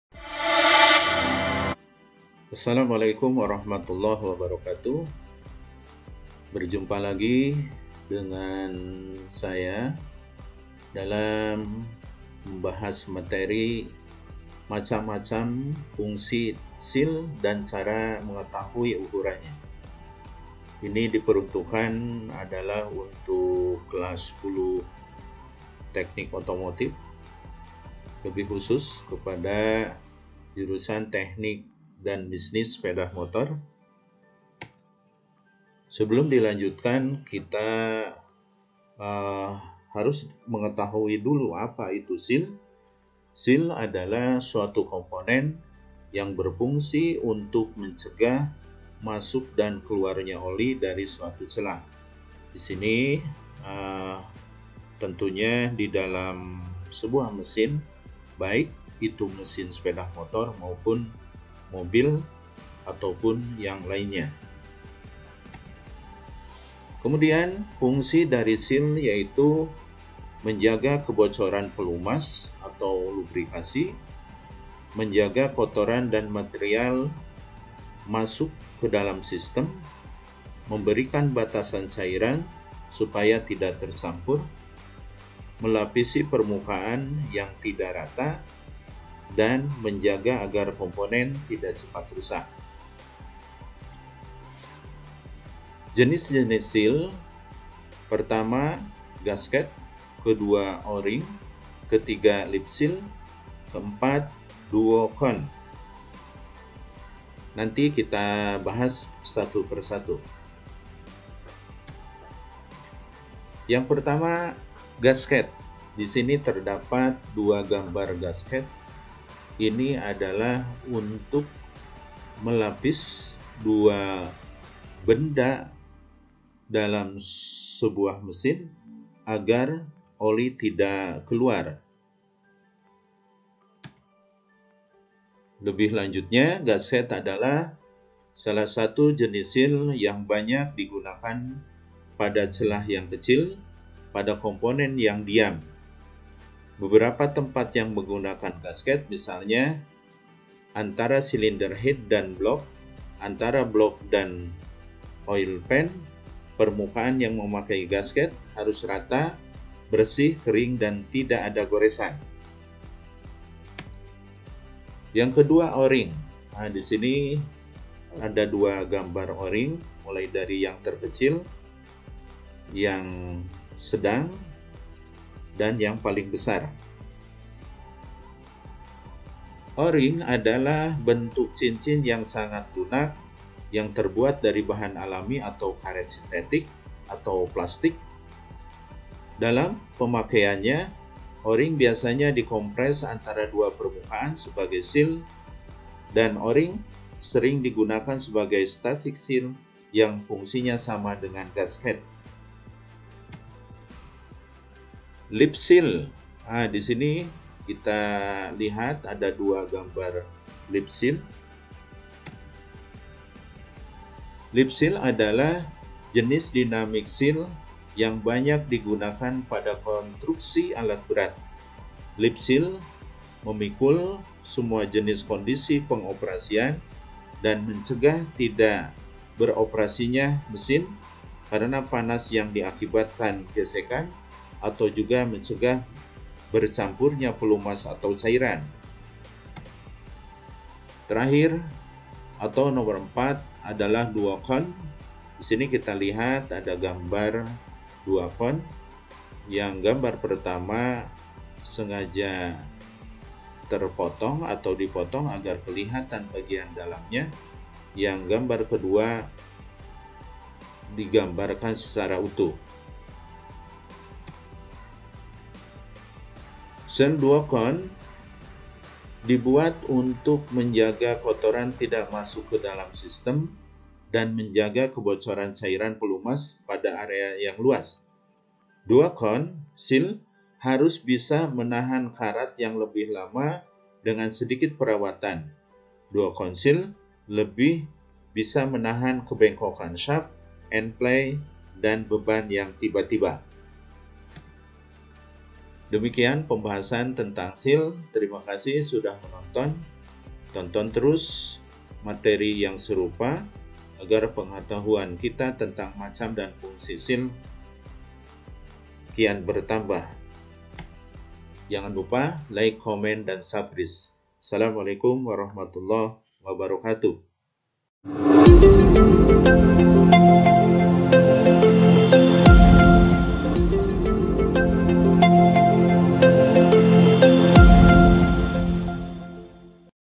Fungsi Seal Dan Jenis-Jenis Seal
Seal adalah suatu komponen yang berfungsi untuk mencegah masuk dan keluarnya oli dari suatu celah. Pemakaian seal dari bahan tertentu disesuaikan tipe perapatannya. Pada mesin, ada 2 jenis perapat, yaitu perapatan statis dan perapatan dinamis.Tipe perapatan statis (static seal) digunakan pada komponen yang tidak bergerak. Sehingga gasket digunakan sebagai bahan antara saat menyambung dua komponen atau lebih. Salah satu atau kedua komponen yang disambung tidak bergerak satu sama lain. Sedangkan bahan pembuatnya disesuaikan dengan karakteristik dari bahan yang disambung tersebut.